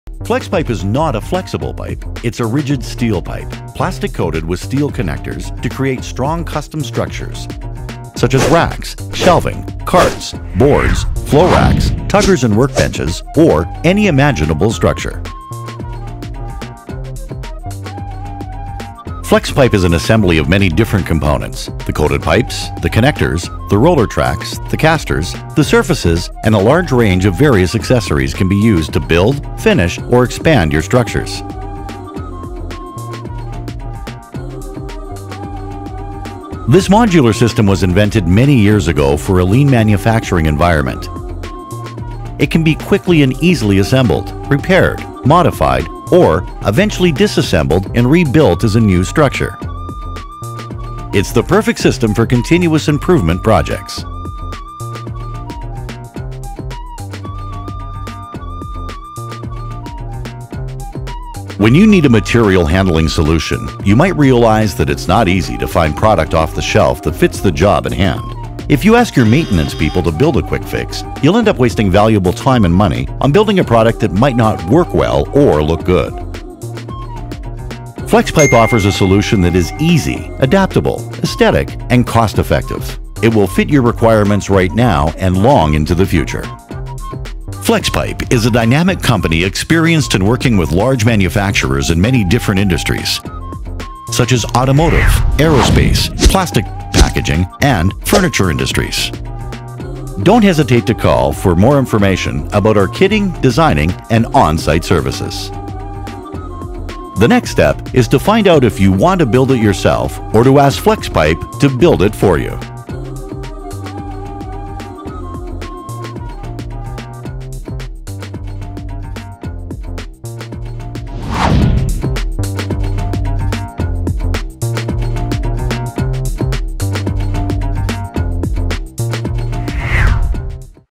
Pipe and joint system for lean manufacturing | Flexpipe
Flexpipe is a manufacturer of a modular pipe and joint system used in a lean manufacturing environment. The metal pipes and connectors are easily assembled together to build industrial carts, workstations, flow racks, KPI boards, and different kitting and handling racks.